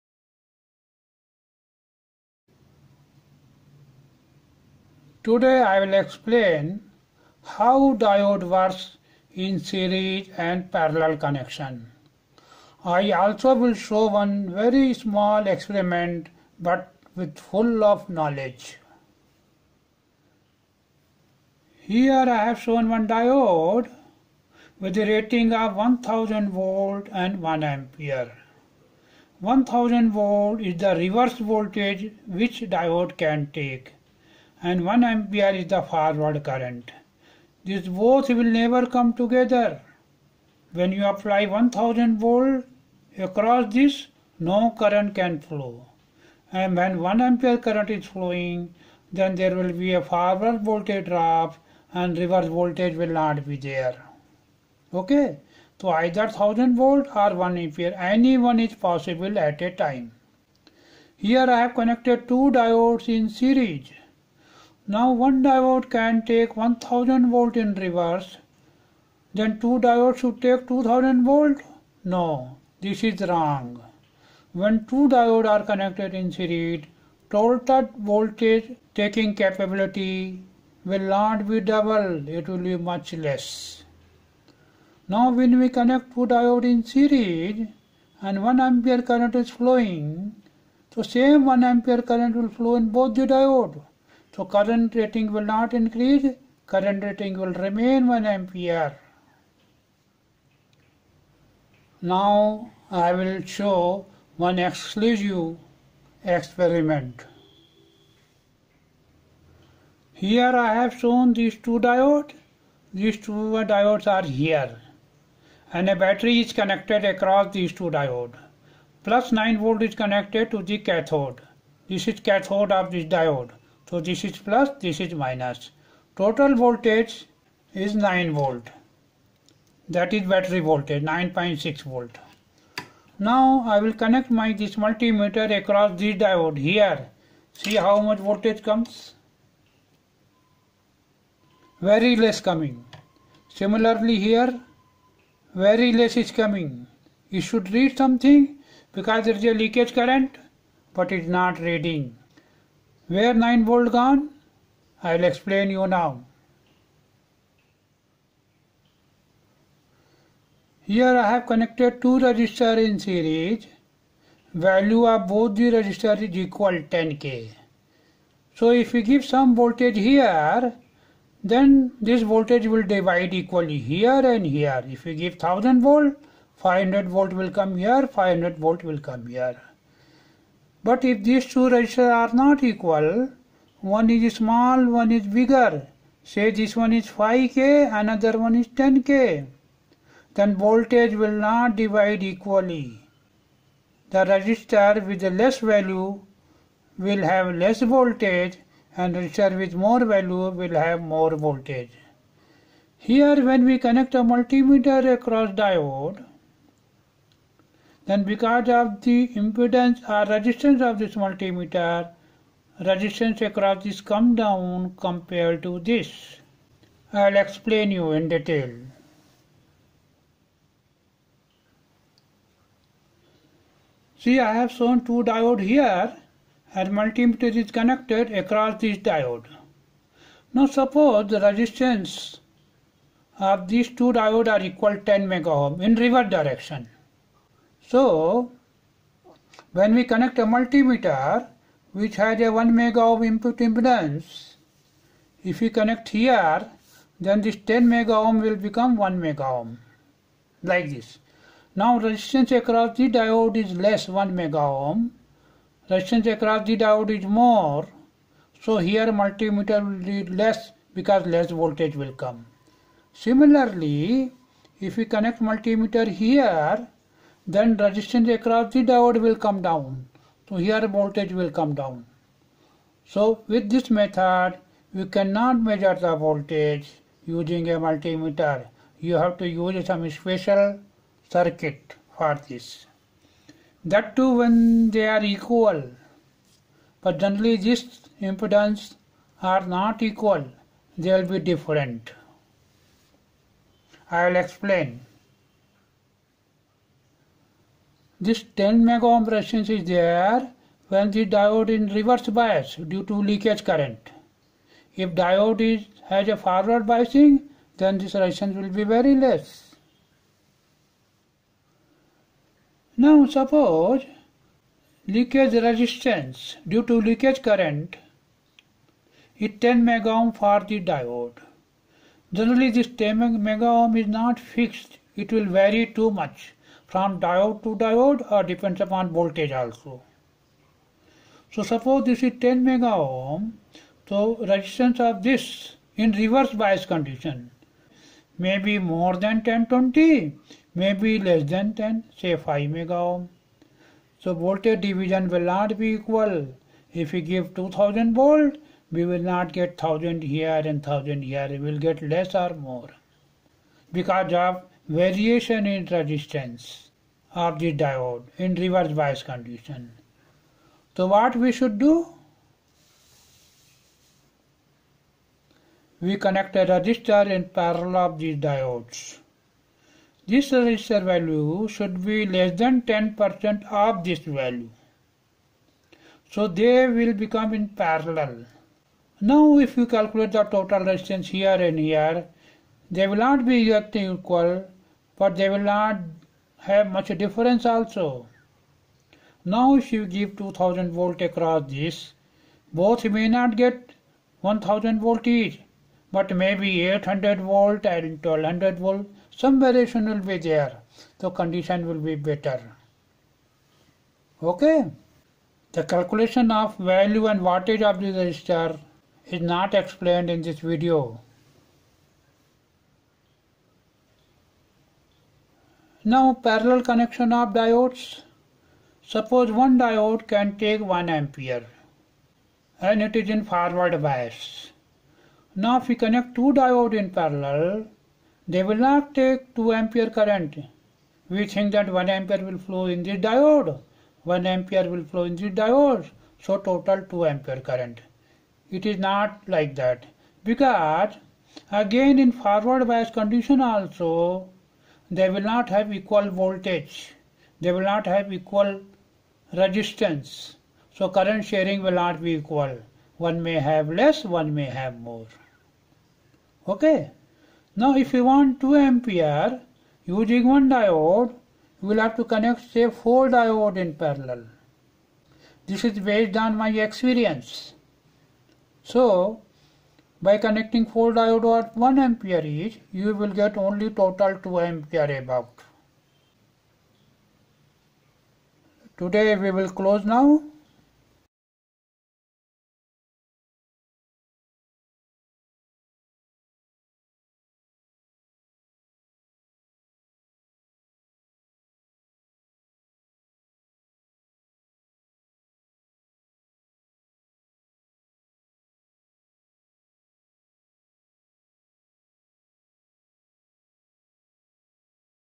Diodes in series and parallel circuit, voltage current calculation
The diode voltage and current calculation in 2 diodes in a series circuit and diode in parallel circuit connection with an experiment is explained in this video. The voltage sharing of diode with two diodes in series is explained. The current sharing in two diodes in parallel connection configuration is explained. An educational tutorial on electrical and electronics engineering 375 by G K Agrawal. The lecture is given by a person with circuit design and industrial experience.

You learned in this video, ampere and voltage sharing in two diodes in series and in parallel connection. The reverse voltage sharing in 2 diodes is series, and current sharing in 2 diodes connected in series is explained.